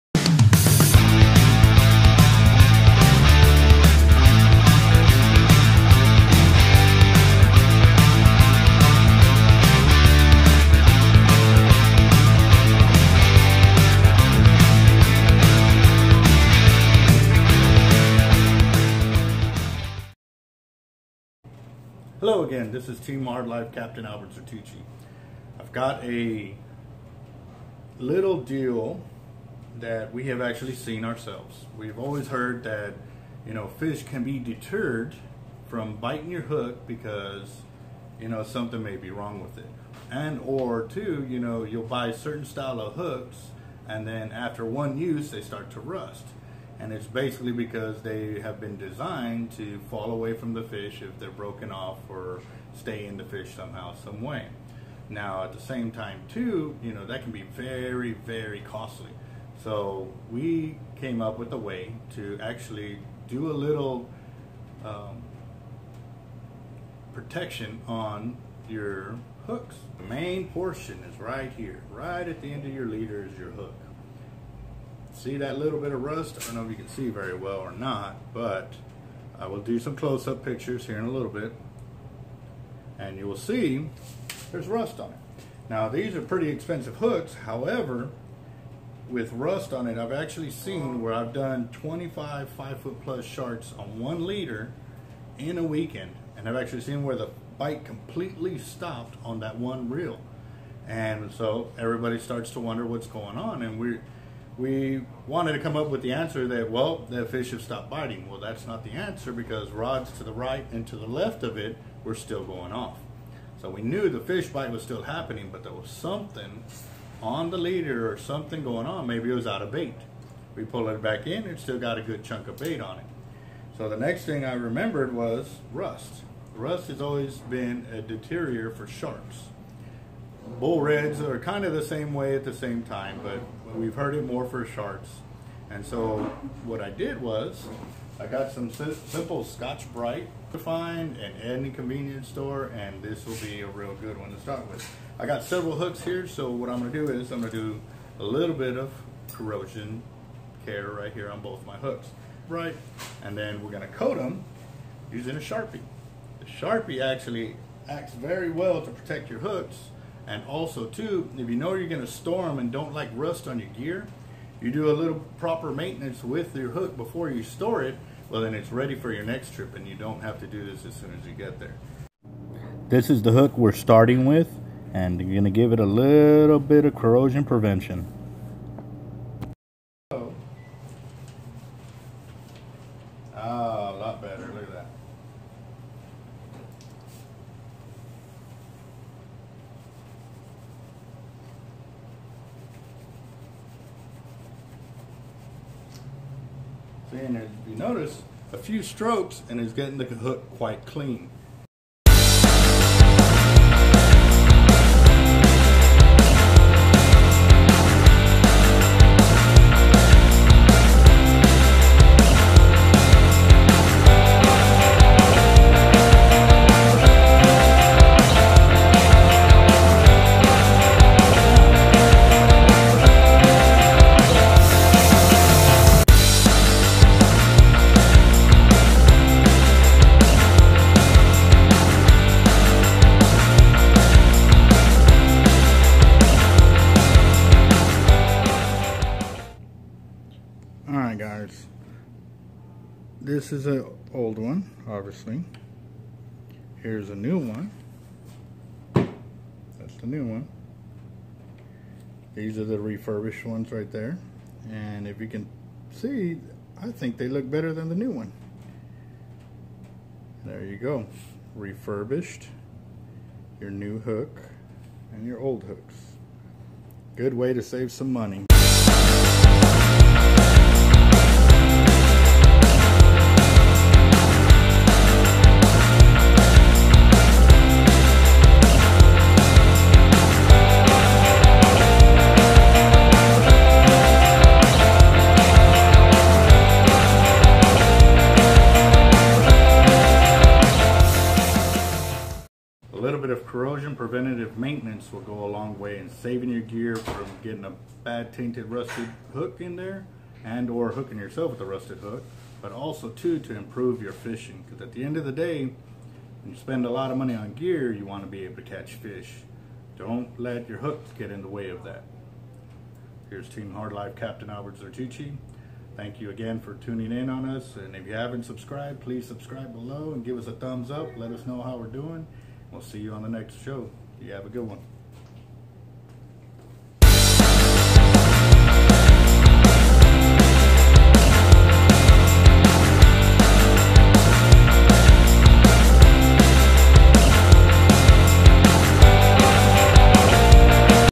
Pro Fishing Hack!!! Do you know how to save money? Yes there are ways to make money go the distance
Pro Fishing Hack!!! Do you know how to save money?

Using simple scotch bright and a magic marker can save you 100s in auto insurance lol... Just kidding ... 100s in savings on buying hooks...